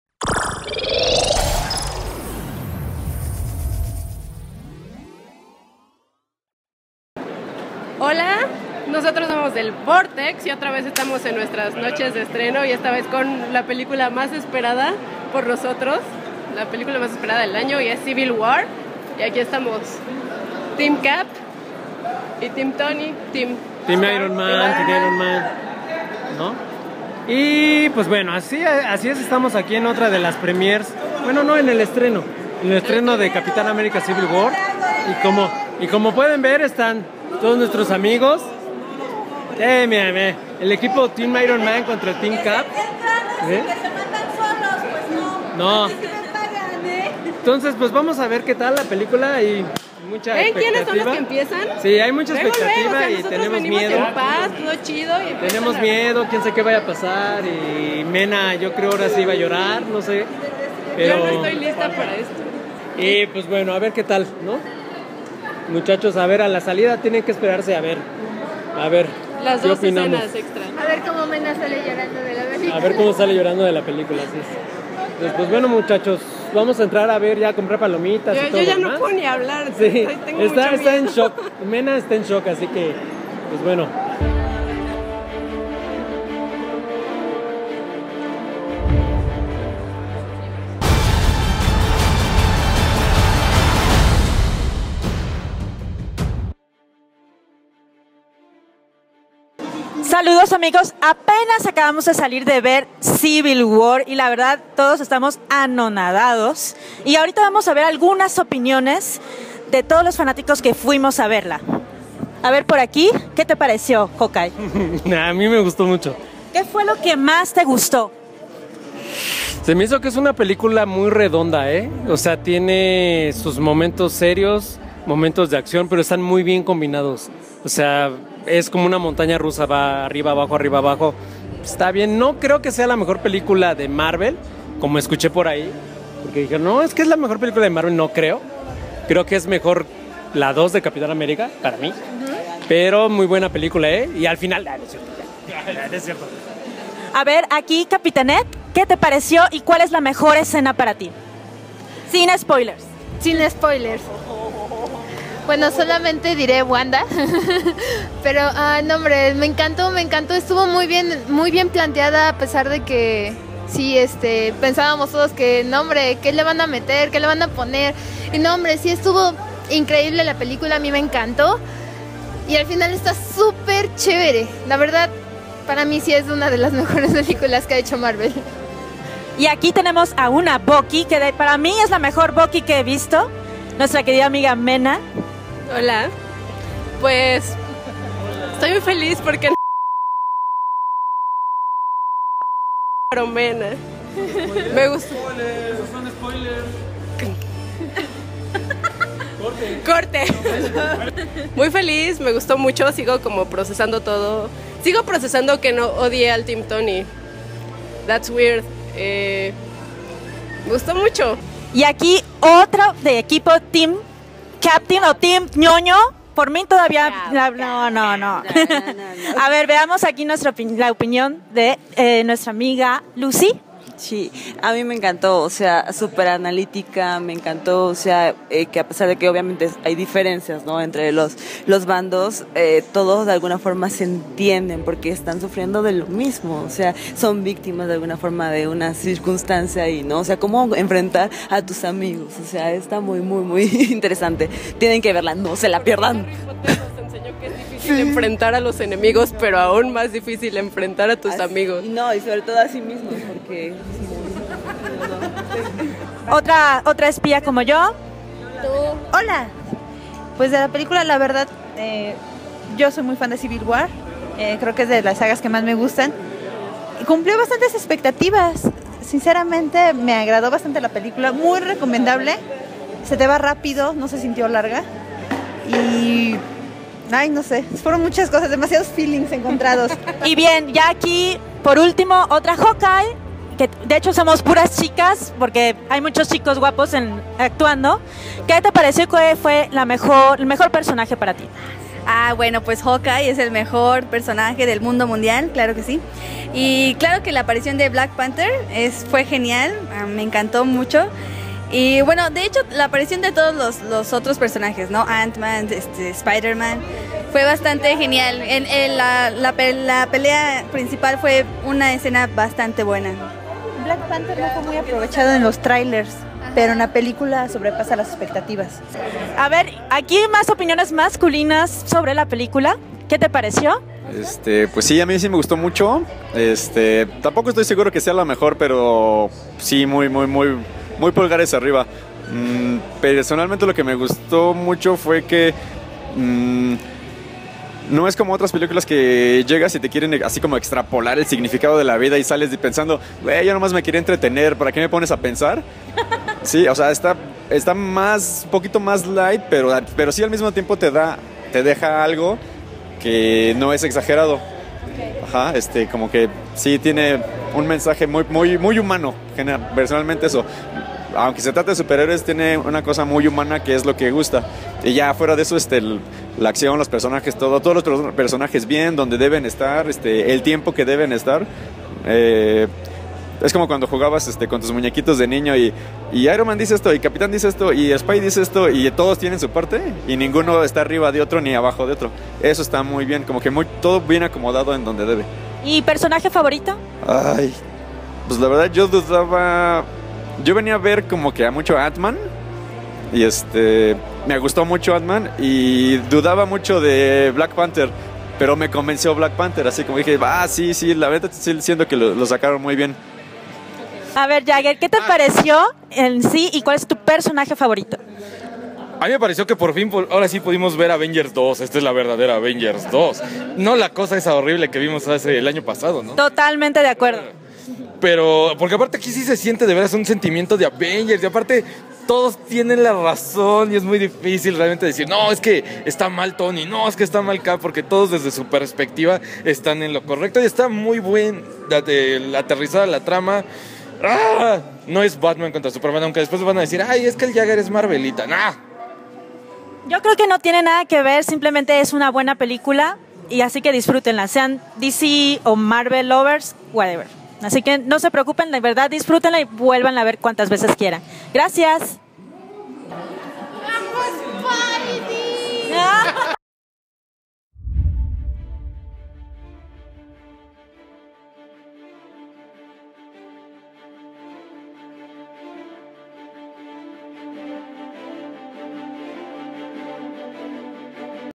El Vortex en el estreno de 'Captain America: Civil War'
EL Vortex y amigos asistieron a la función de estreno de Capitán América: Civil War y nos dan sus primeras impresiones y reacciones a la película más esperada del año.